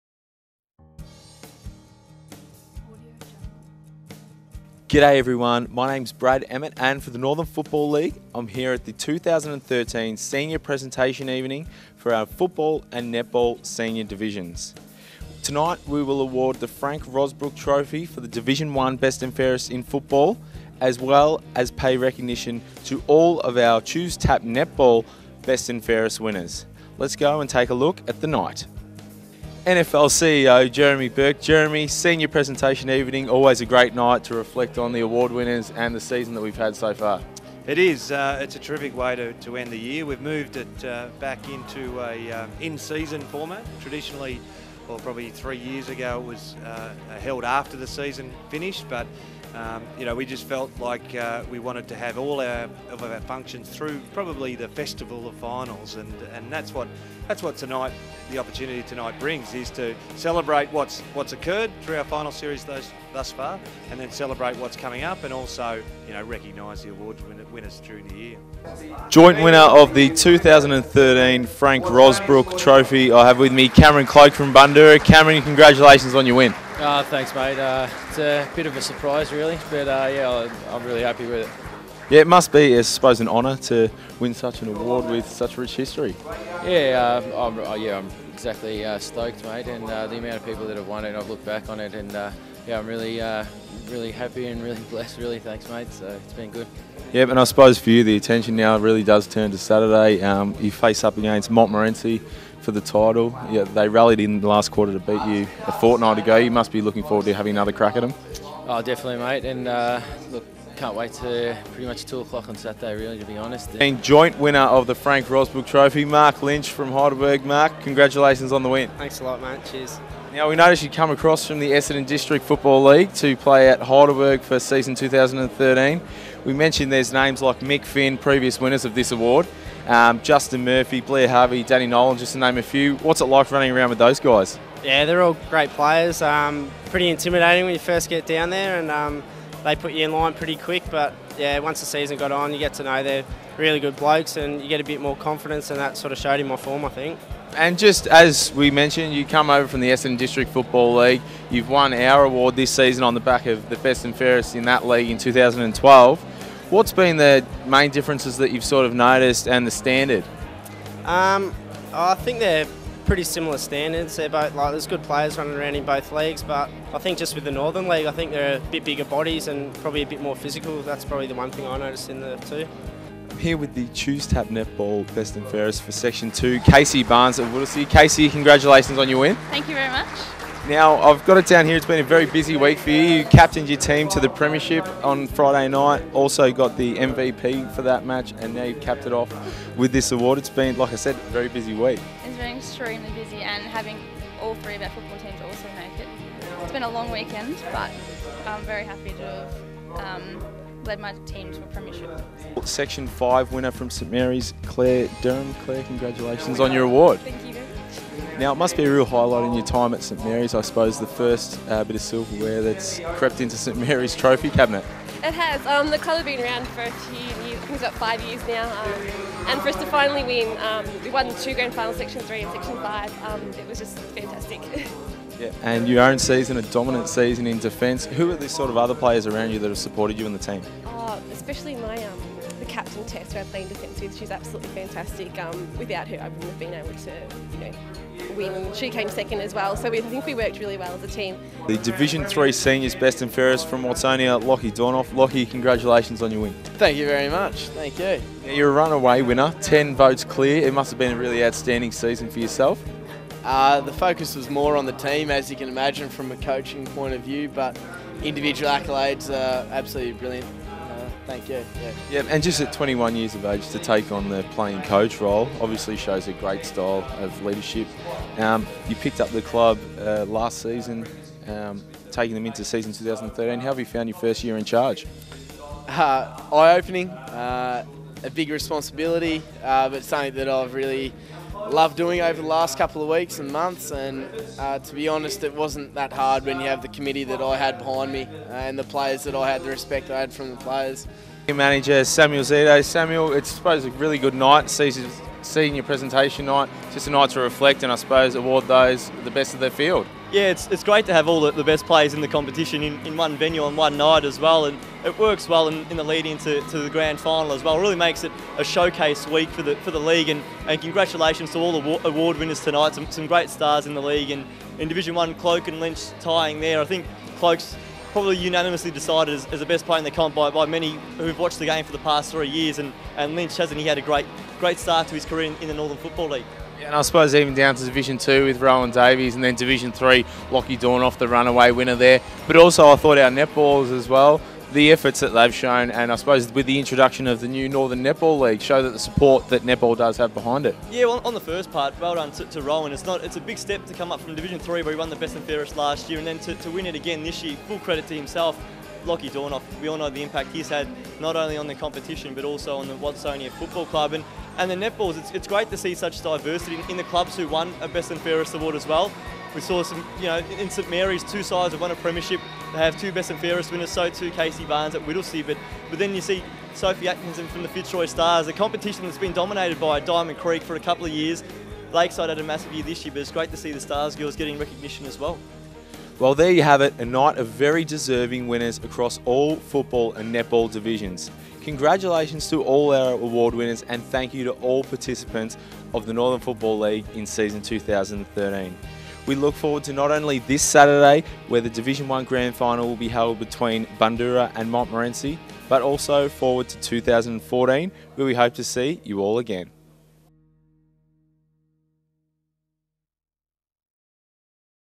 NFL Senior Presentation Evening - 2013
NFL TV reporter Brad Emmett takes us through the Northern Football League Senior Presentation Night for 2013. Watch as he chats to some of the Best and Fairest winners across both football and netball.